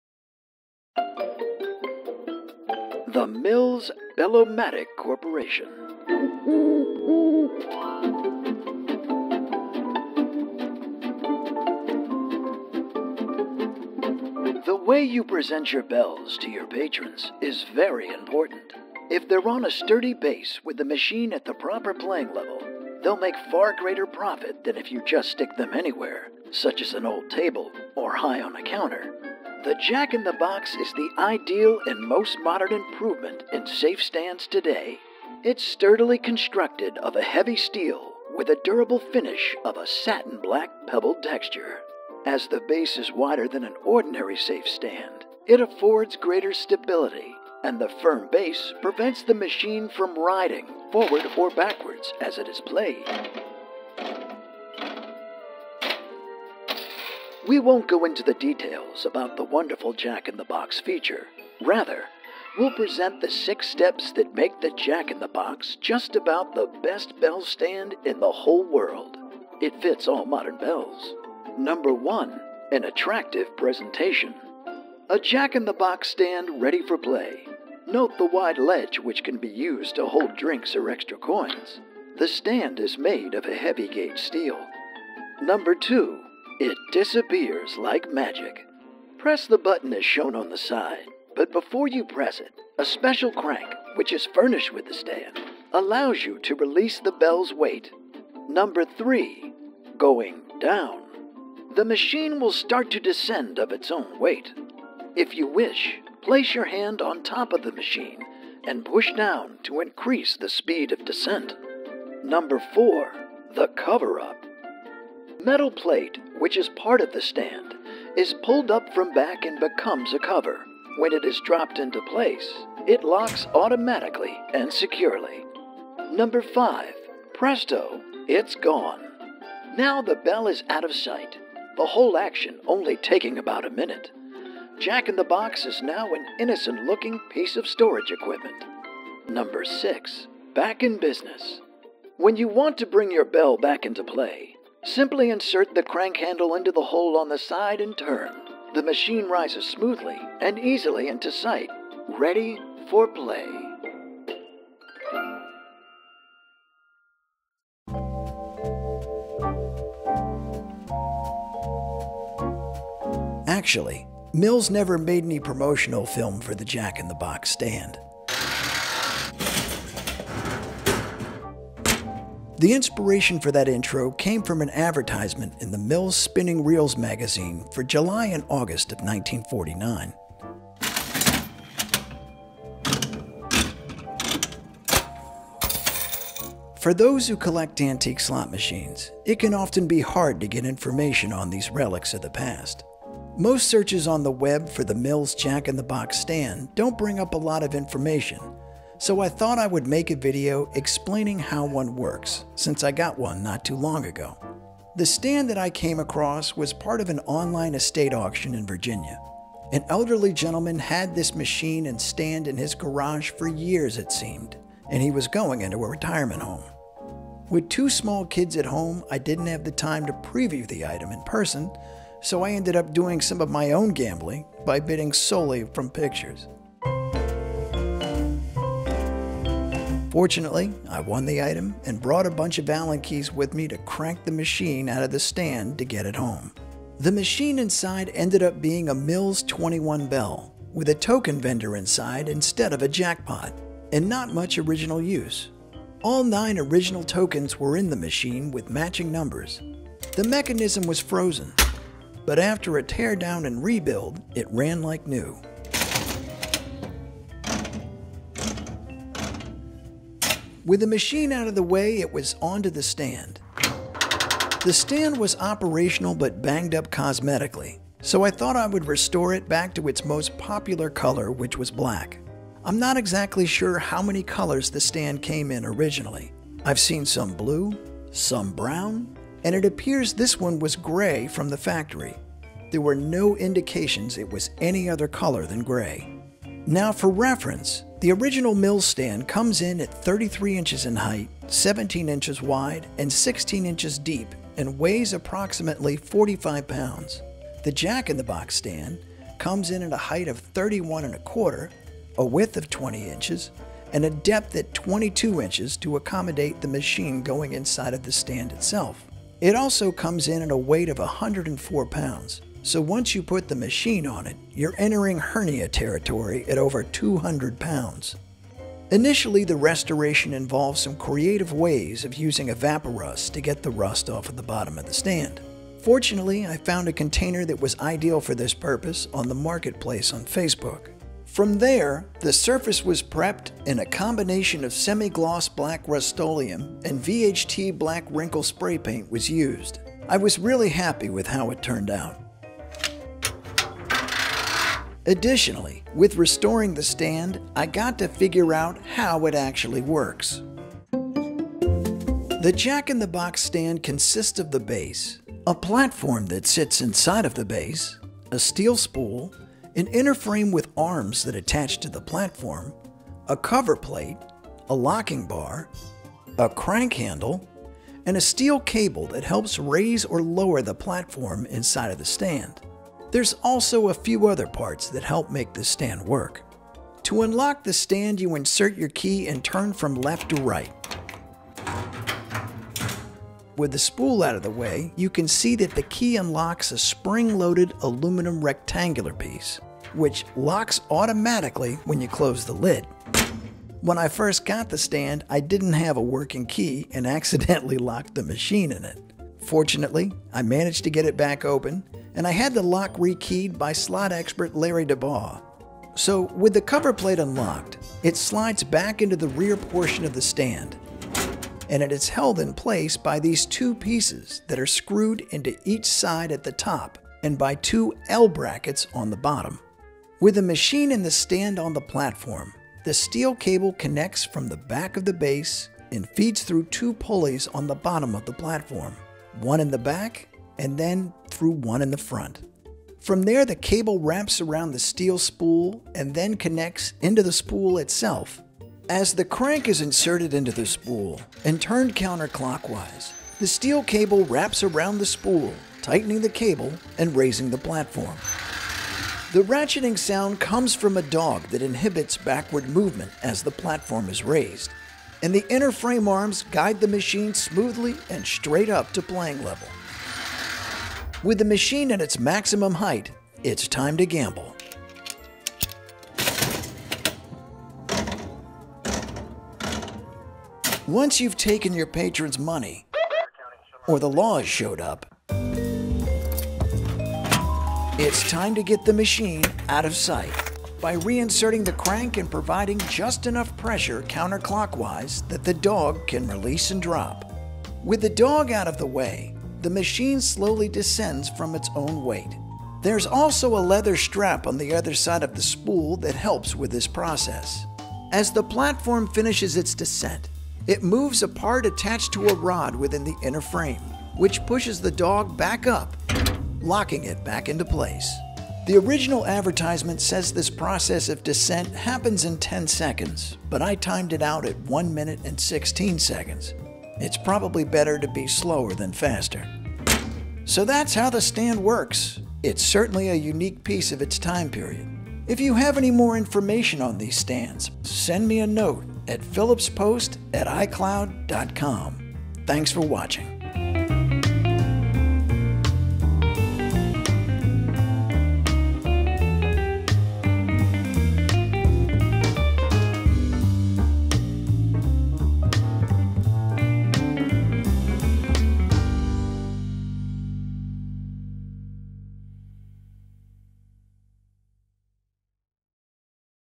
The Mills Jack-In-The-Box Slot Machine Stand
This video goes over a unique antique from the late 1940's through the early 1950's.  The Jack In the Box Stand allowed owners to hide the machine within the stand itself in various gambling establishments, particularly those that weren't supposed to have gambling!  There's not a lot of information on these stands on the internet so this video provides insight into how one works.